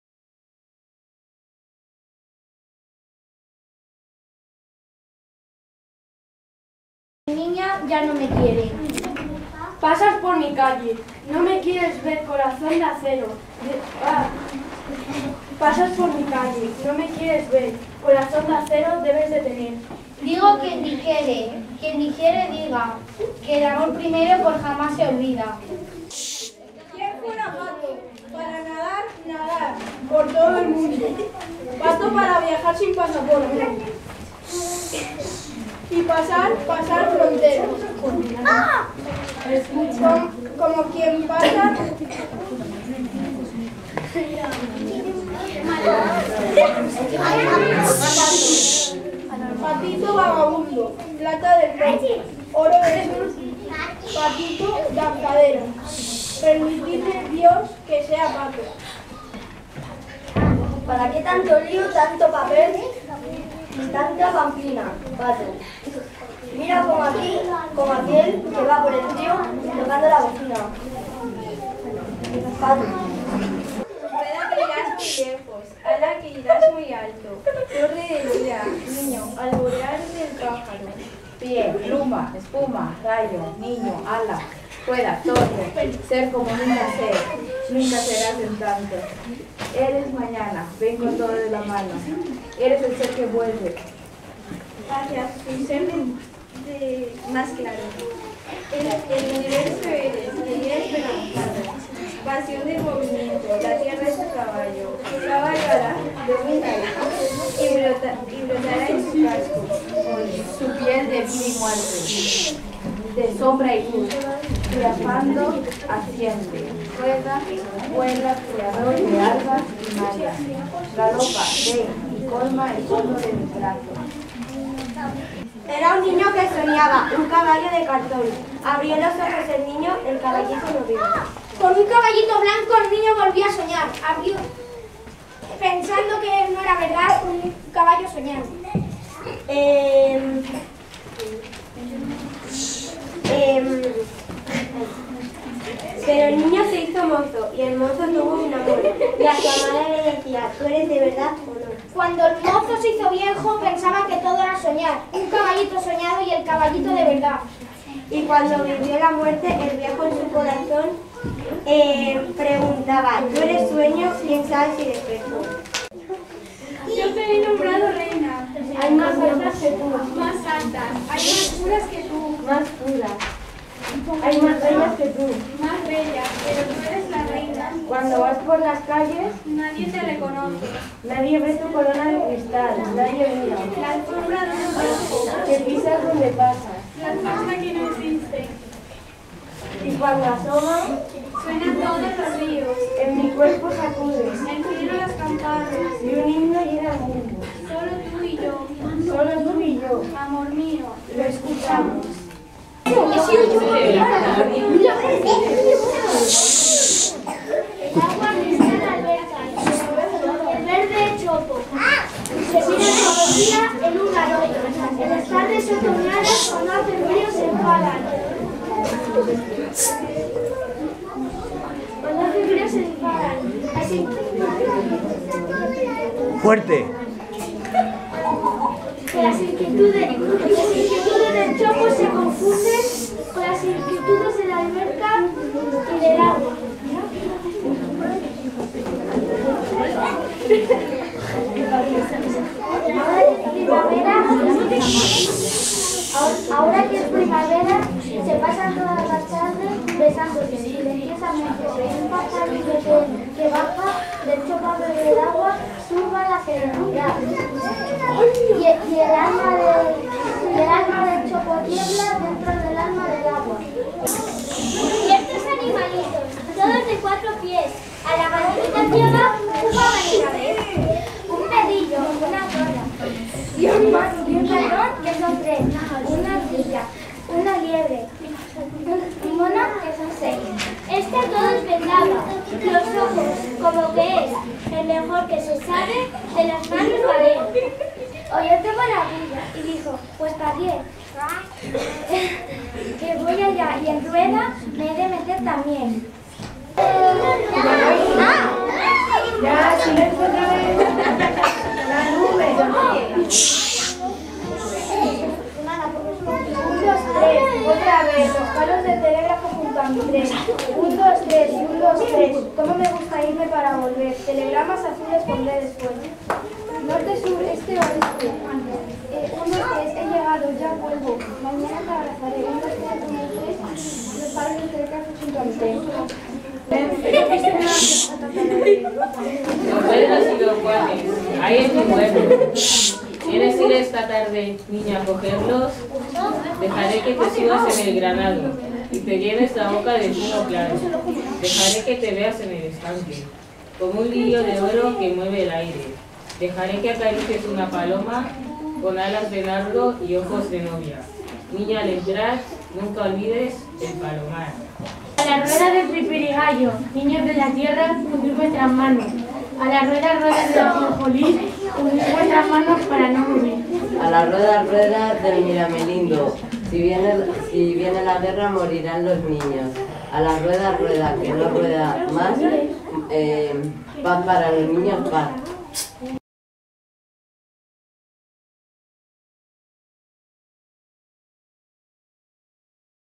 Poemas dramatizados para infantil
Los alumnos de 6º B del CEIP Maria Montessori de Móstoles dramatizan 10 poemas y lo representan con luz negra para los alumnos de Educación Infantil del colegio.